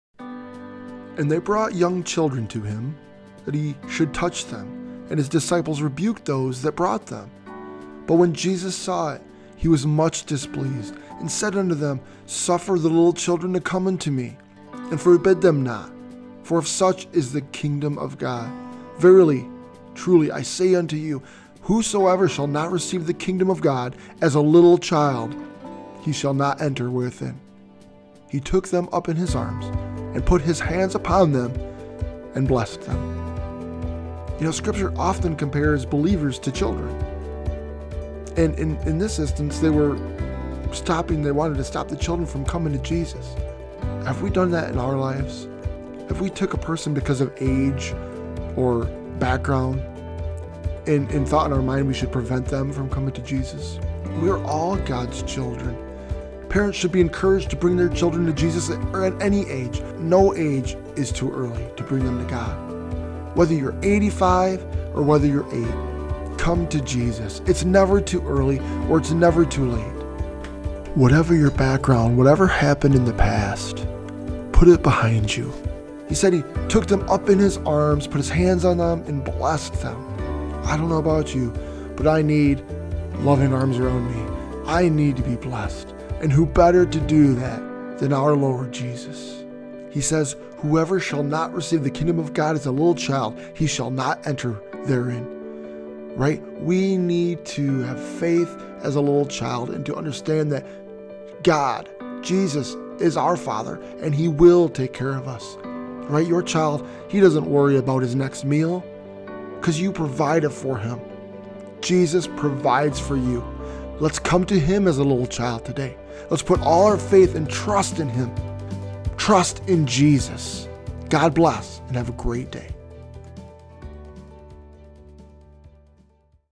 Let the Children Come to Me! Let Jesus take you up in his Arms today!
Mar 10:13  And they were bringing children to Him so that He would touch them; but the disciples rebuked them.
Mar 10:14  But when Jesus saw this, He was indignant and said to them, “Allow the children to come to Me; do not forbid them, for the kingdom of God belongs to such as these.
Mar 10:15  “Truly I say to you, whoever does not receive the kingdom of God like a child will not enter it at all.”
Mar 10:16  And He took them in His arms and began blessing them, laying His hands on them.

Read from NASB